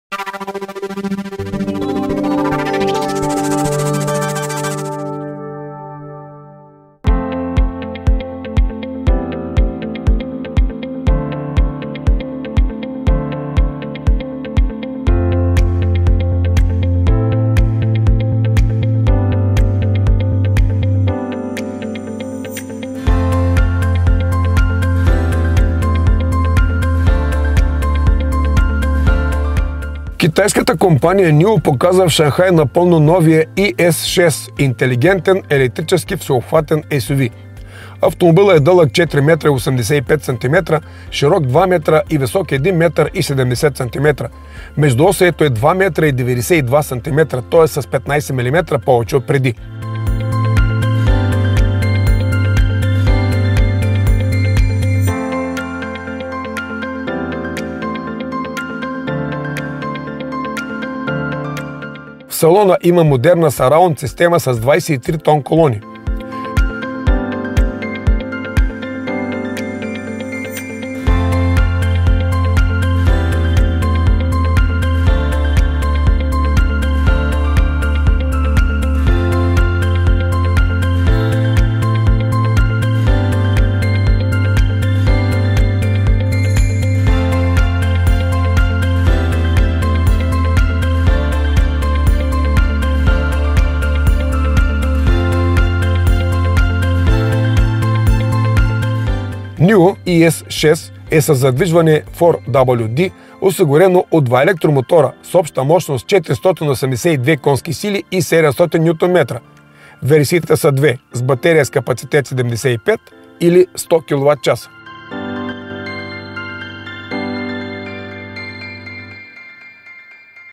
Auto Show Shanghai 2023 Nio ES6
Китайската компания Nio показа в Шанхай напълно новия ES6 - интелигентен електрически всеобхватен SUV. Автомобилът е дълъг 4 м и 85 см, широк 2 м и висок 1 м и 70 см. Междуосието е 2 м и 92 см, т.е. с 15 мм повече от преди. В салона има модерна съраунд система с 23 тонколони. Nio ES6 e със задвижване 4WD осигурено от два електромотора с обща мощност 482 к.с. и 700 Нм ВМ. Версиите са две: с батерия с капацитет 75- или 100 кВт/ч.